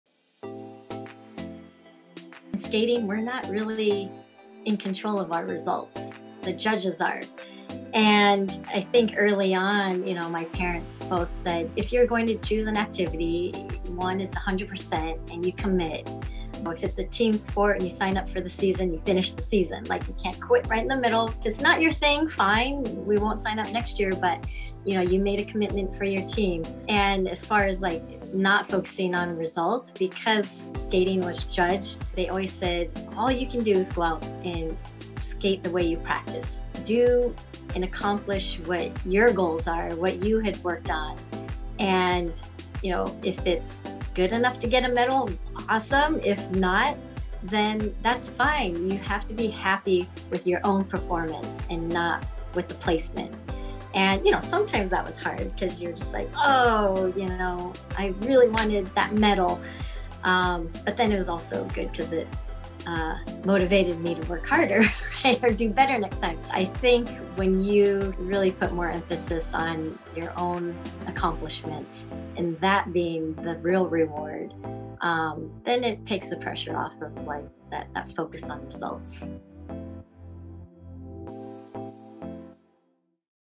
Advice from Kristi Yamaguchi - Asian Heritage Month
Huge shoutout to Kristi Yamaguchi for taking the time to inspire Autodesk Asian Network in honor of AsianHeritageMonth! Check out this video to hear some of her gold medal advice! 🥇